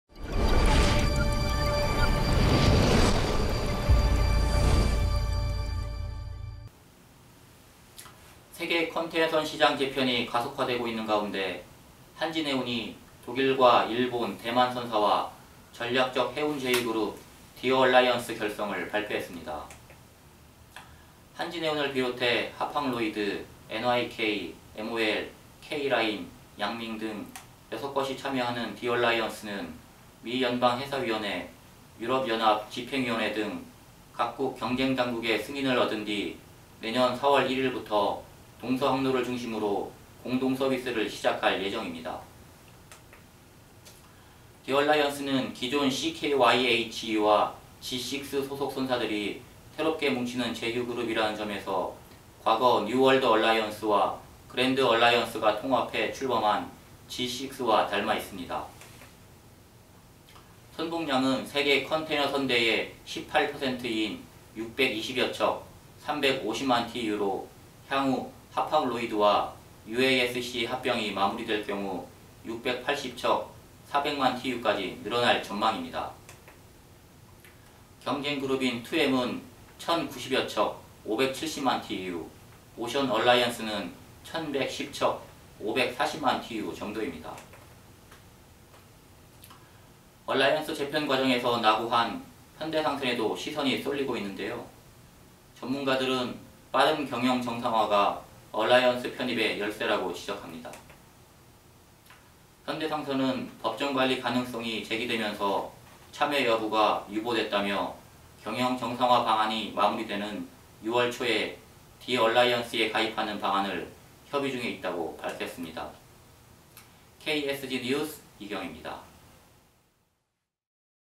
'얼라이언스 재편 마감' 한진해운 현대상선 희비 교차
세계 컨테이너선 시장 재편이 가속화되고 있는 가운데 한진해운이 독일과 일본 대만 선사와 전략적 해운제휴그룹 디얼라이언스 결성을 발표했습니다.

한진해운을 비롯해 하파그로이드 NYK MOL 케이라인 양밍 등 6곳이 참여하는 디얼라이언스는 미연방해사위원회 유럽연합집행위원회 등 각국 경쟁당국의 승인을 얻은 뒤 내년 4월1일부터 동서항로를 중심으로 공동 서비스를 시작할 예정입니다.

디얼라이언스는 기존 CKHYE와 G6 소속선사들이 새롭게 뭉치는 제휴그룹이라는 점에서 과거 뉴월드얼라이언스와 그랜드얼라이언스가 통합해 출범한 G6와 닮아 있습니다.

선복량은 세계 컨테이너선대의 18%인 620여척 350만TEU로, 향후 하파그로이드와 UASC의 합병이 마무리될 경우 680척 400만TEU까지 늘어날 전망입니다. 경쟁그룹인 2M은 1090여척 570만TEU, 오션얼라이언스는 1110척 540만TEU 정도입니다.

얼라이언스 재편 과정에서 낙오한 현대상선에도 시선이 쏠리고 있는데요, 전문가들은 빠른 경영정상화가 얼라이언스 편입의 ‘열쇠’라고 지적합니다.

현대상선은 올 초부터 법정관리 가능성이 제기되면서 참여 여부가 ‘유보’된 것으로 경영정상화 방안이 마무리 되는 6월 초에 디얼라이언스에 가입하는 방안을 협의 중에 있다고 밝혔습니다.